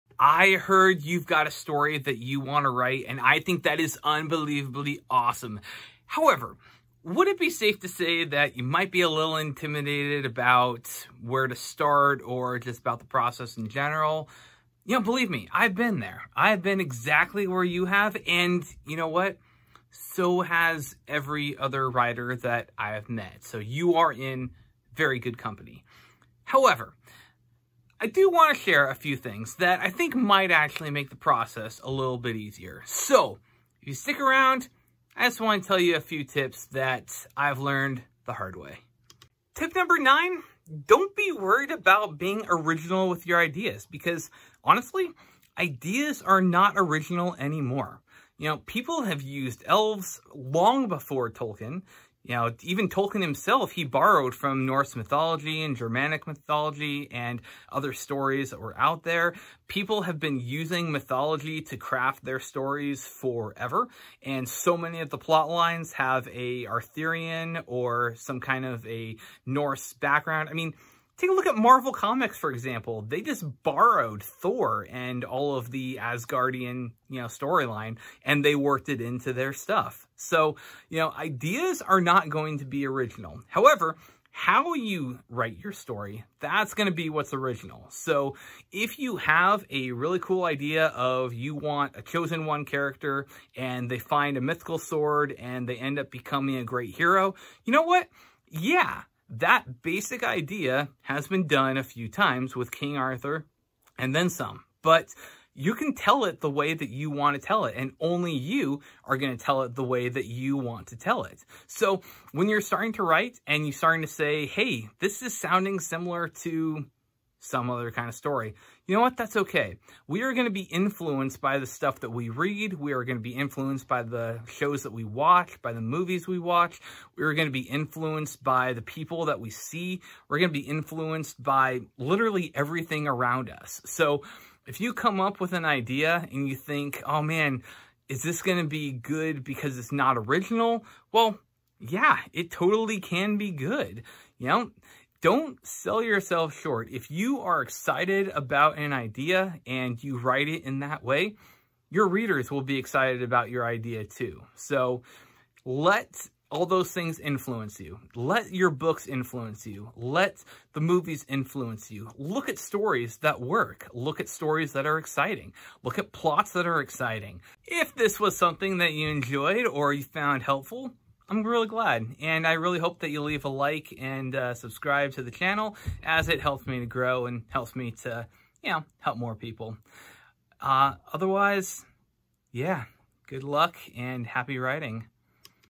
Writing Tips 9/10 - Don't be worried about your ideas not being original
I heard you’ve got a story you want to write... That is so cool!

However, would it be safe to say you might be a little intimidated by the process, or even just where to begin? I’ve been there, I get you, and you know what? Pretty much every writer has been in exactly the same place as you, so you are in good company.

Since I have been where you are, I just wanted to share a few things that might help, a few writing tips if you will, so, here are ten things new writers need to know if they want to write a book.

Yes, this is part 9, which means there is one more on the way if you'd like to hear it. This is about what to do when you feel your ideas aren't original, and how to deal with it.

If you're interested in my own books, I write fantasy, as well as books about writing fantasy, and you can find them all here:

Time Stamp:
Intro - 00:00
Tip 9 - 00:39
Closing -  02:47